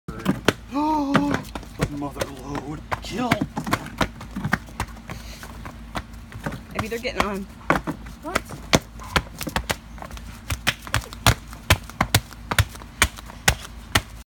Earwig Mother Load
Dad spends quality time with daughter squashing earwigs. The Mother load!